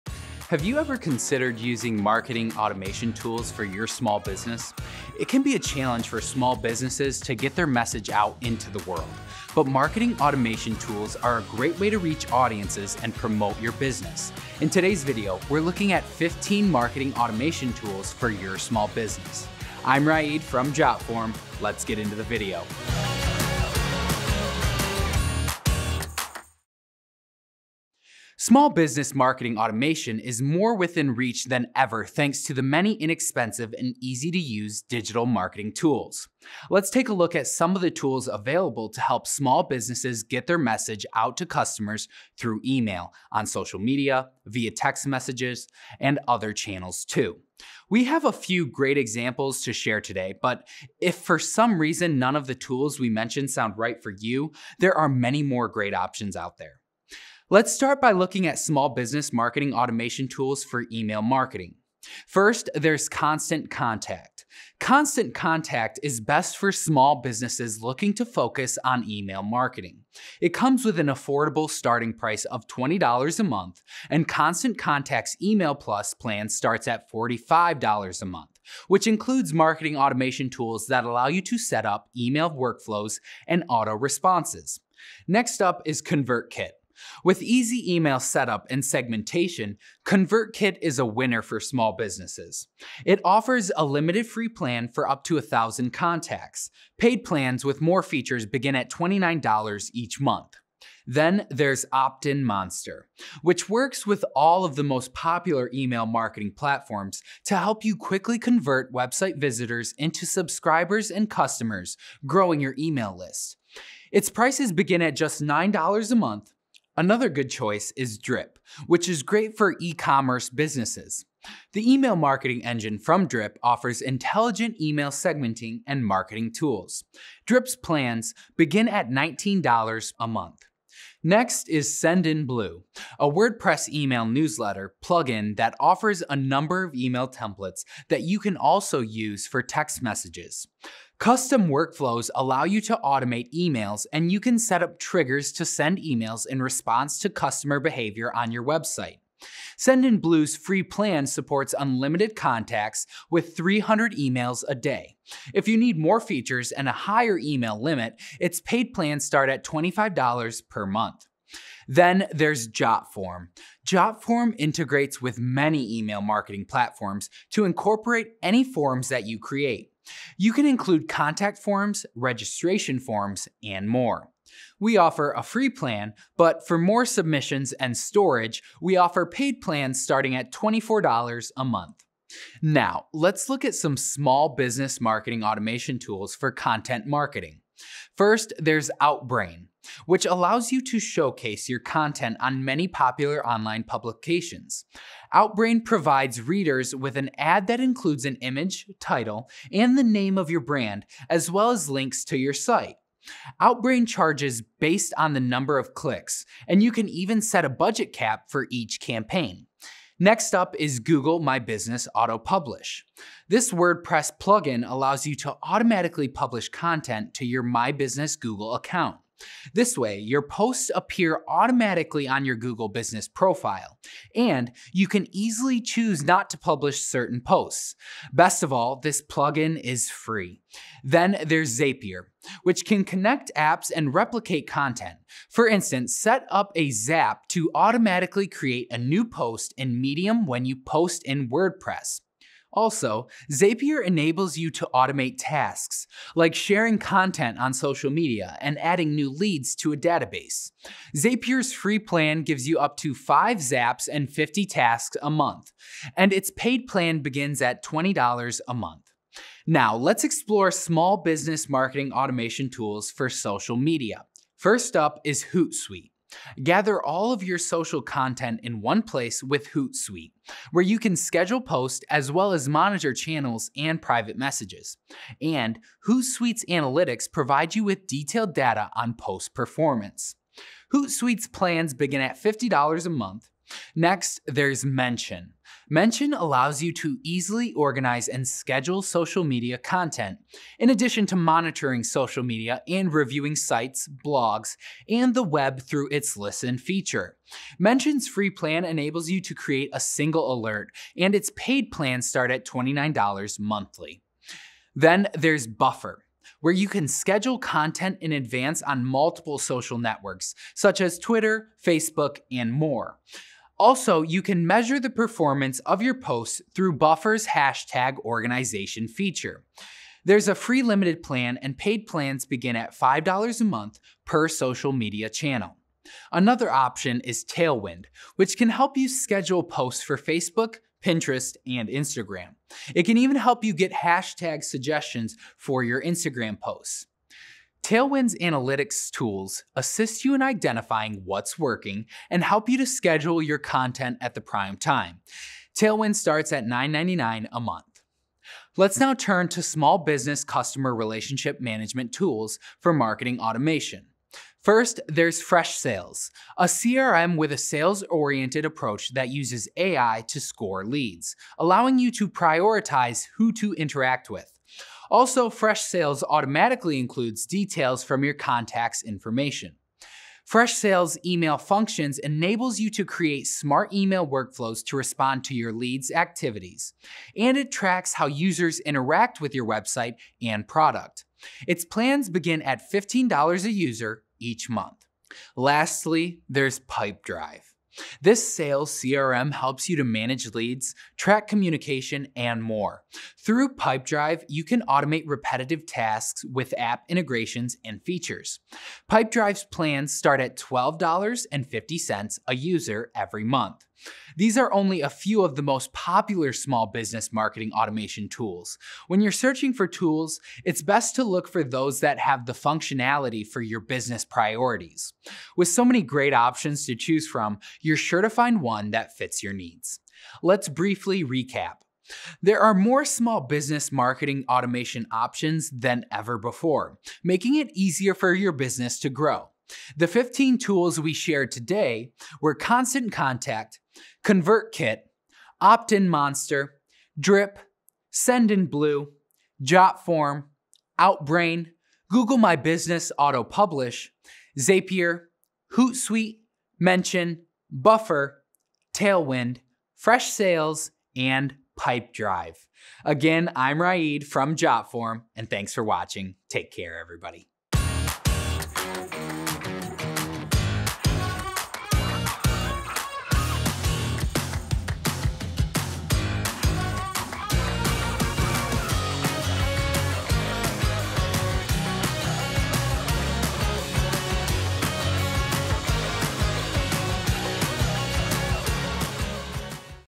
Top 15 Small Business Marketing Automation Tools
Discover how easy it is to improve your marketing workflow with powerful automation tools. Join us as we explore some of the top small business marketing automation tools on the market — and weigh their benefits and features to help you find the perfect fit for your needs.
LINKS & RESOURCES
👋 ABOUT JOTFORM
Hi, we're Jotform, a full-featured online forms platform that makes it easy to create robust forms and collect important data. Check us out
00:00 Introduction
00:28 Best marketing automation for small business
01:03 Constant Contact
01:26 ConvertKit
01:41 OptinMonster
01:58 Drip
02:12 Sendinblue
02:43 Jotform
03:09 Outbrain
03:31 Google my business auto publish
03:52 Zapier
04:25 Hootsuite
04:45 Mention
05:05 Buffer
05:27 Tailwind
05:54 Freshsales
06:27 Pipedrive
07:10 Recap
07:34 Subscribe to Jotform